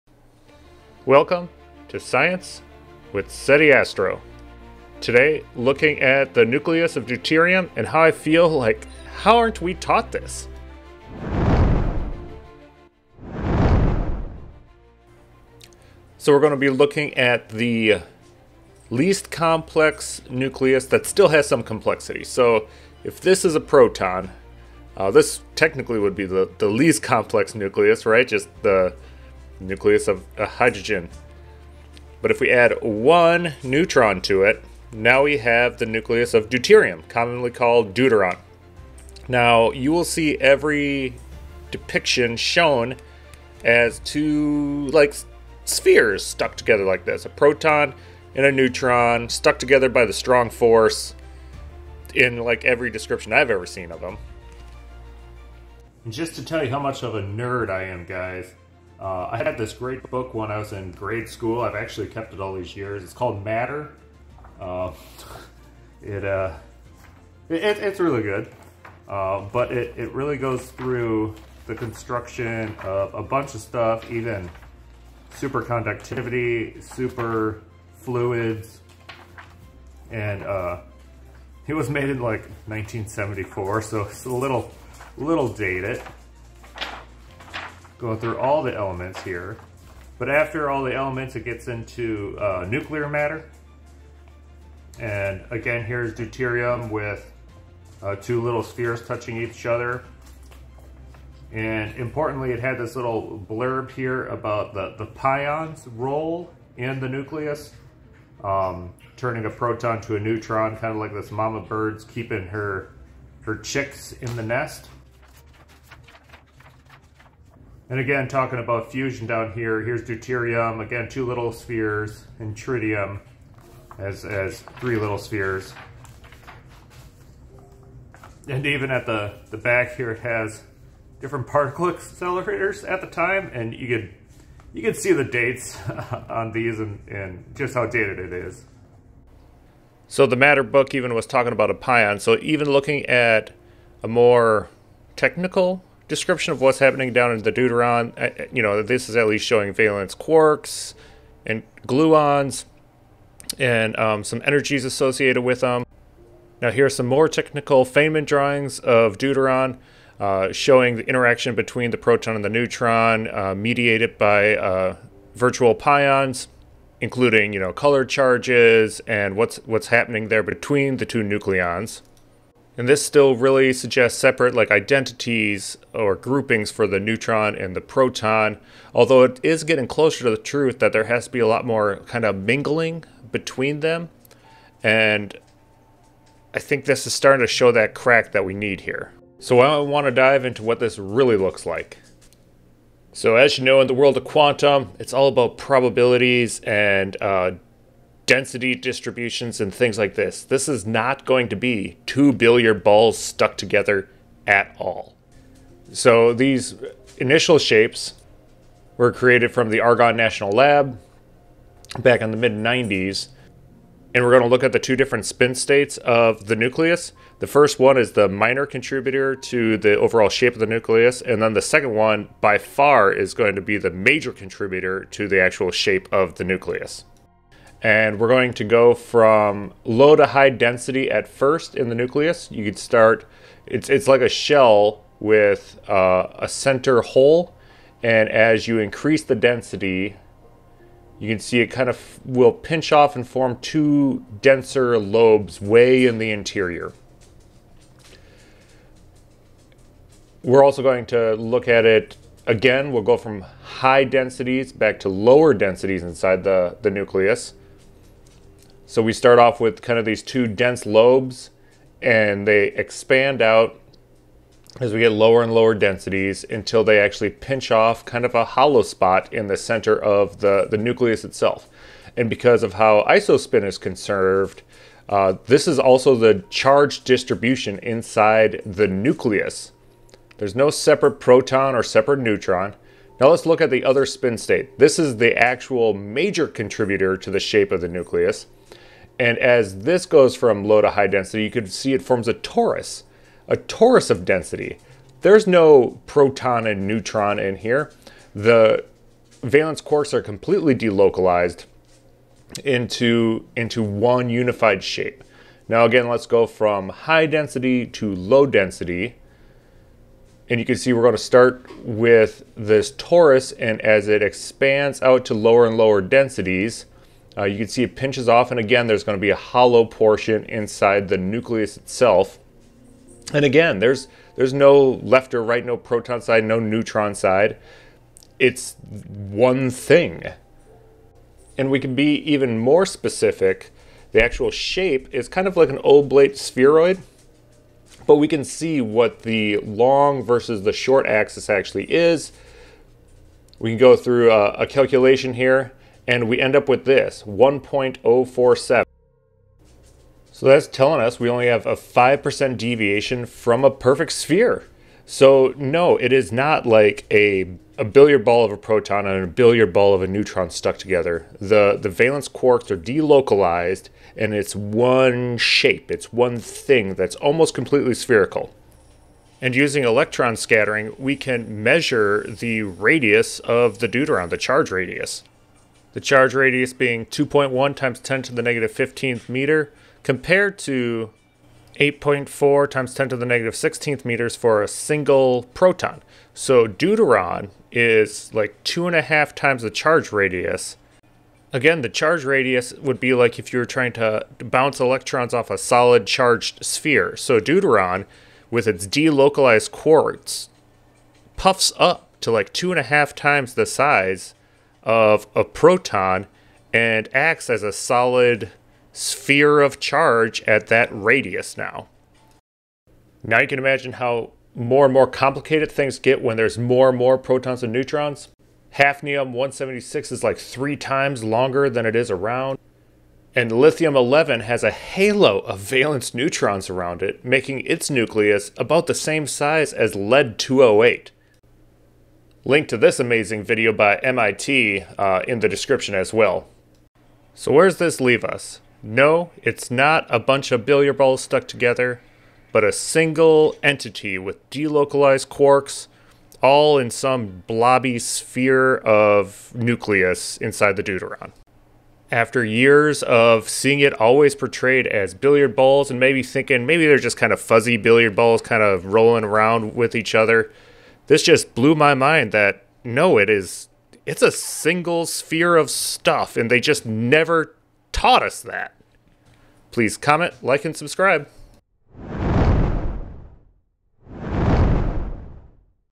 True Shape of a Nucleus: Science Sunday with Seti Astro
In this episode, a few years ago I was completely taken aback when I found out the true shape of the deuterium nucleus.  I am definitely not unfamiliar with nuclear descriptions, feynman diagrams, etc and just couldn't believe we aren't taught what the true shape of some of these nuclei are!